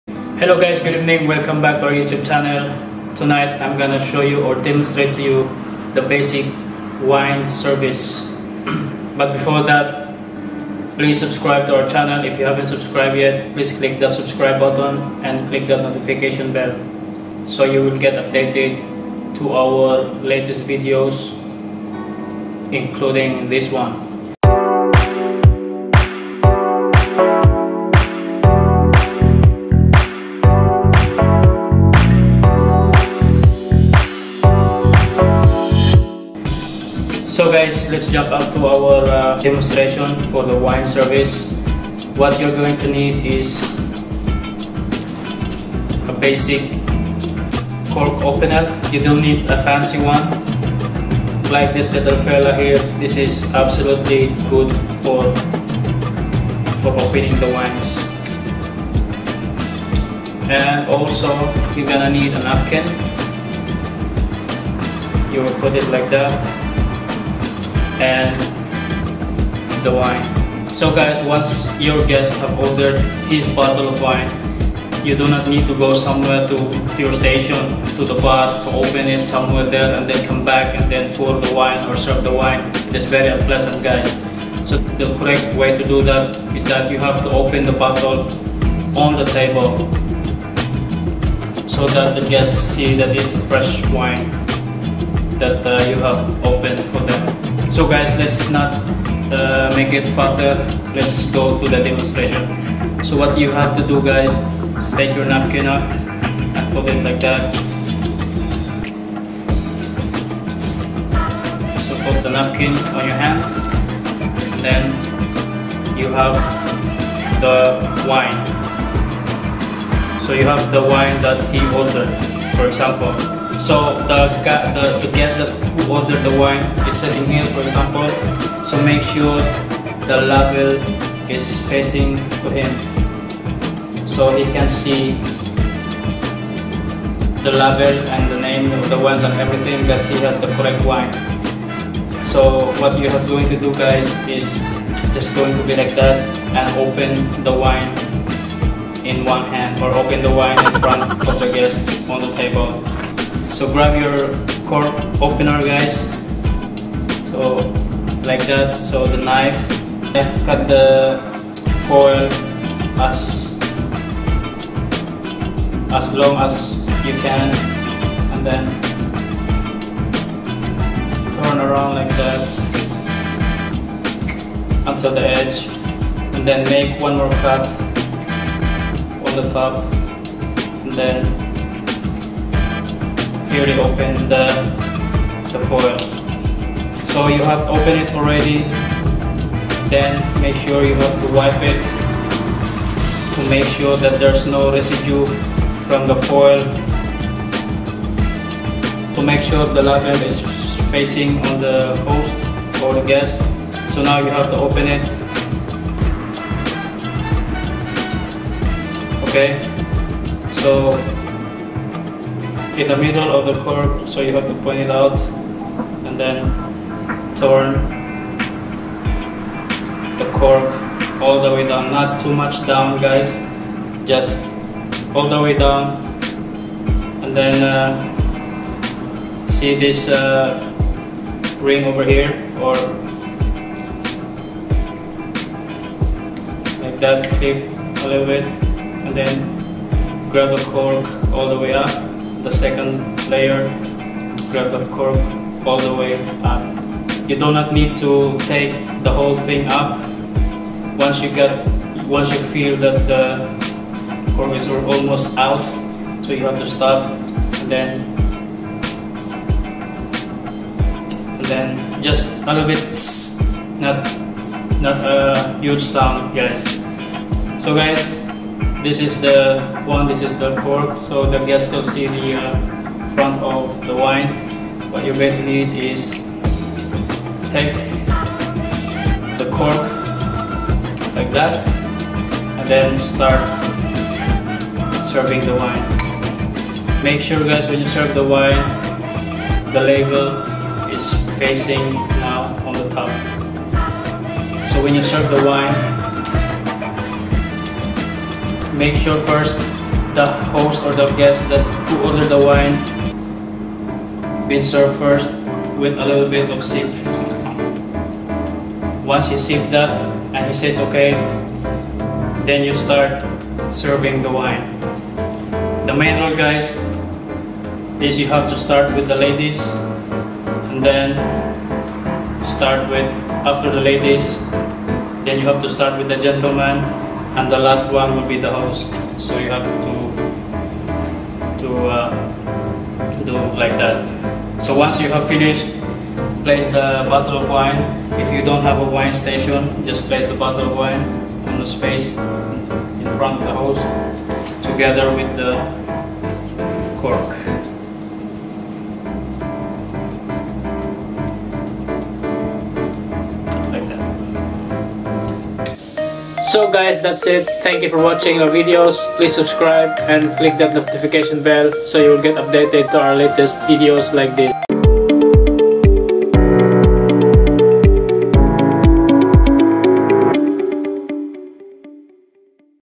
How to serve a bottle of wine F&B tips
A demonstration on astandard wine service,on how to serve the wine to your guest.

If the wine needs to be chilled a wine bucket is required in the table or in a wine station {small table beside in the guest table}.
But mostly it is drink in a room temperature.